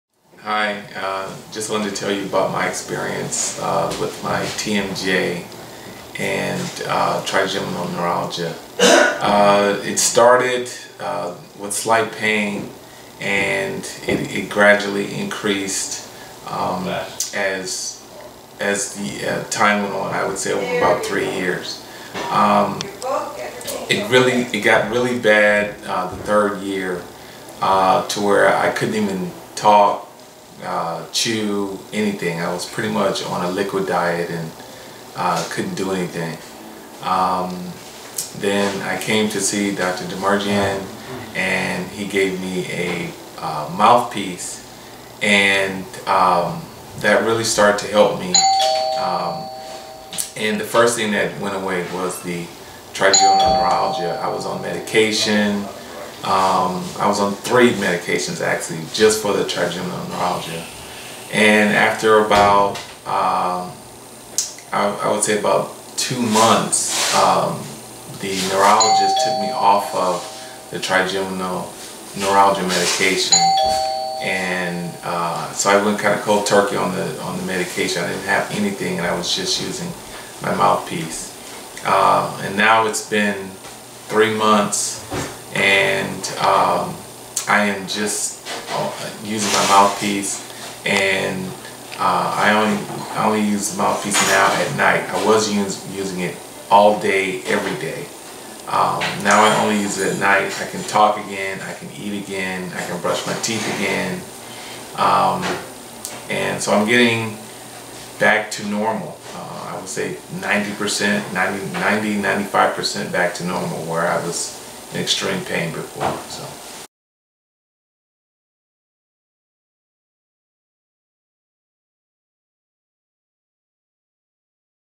Trigeminal Neuralgia & TMJ Connection
Suffering with Trigeminal Neuralgia for 3 years.  He couldn't talk or chew without having pain.  He was on 3 medications and was taken off his meds. after a few months of being on the mouth piece.